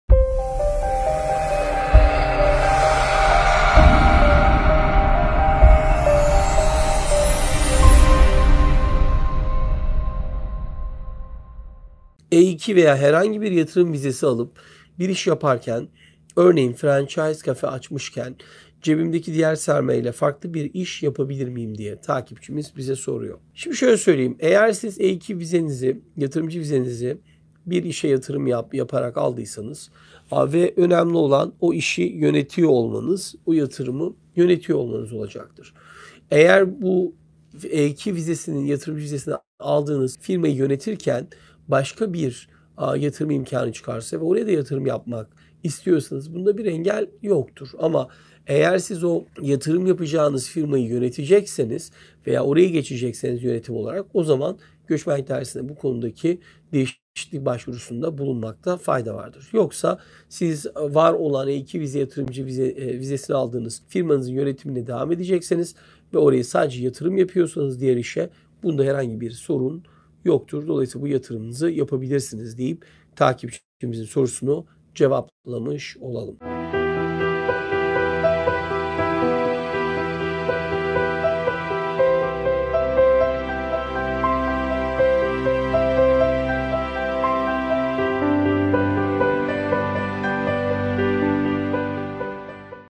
AMERİKA’DA E2 VİZESİYLE YATIRIM YAPIP İKİNCİ BİR İŞ YAPILIR MI ?
Soru: E2 veya herhangi bir yatırım vizesi alıp bir iş yaparken örneğin franchise cafe açmışken, cebimdeki diğer sermaye ile farklı bir iş yapabilir miyim ?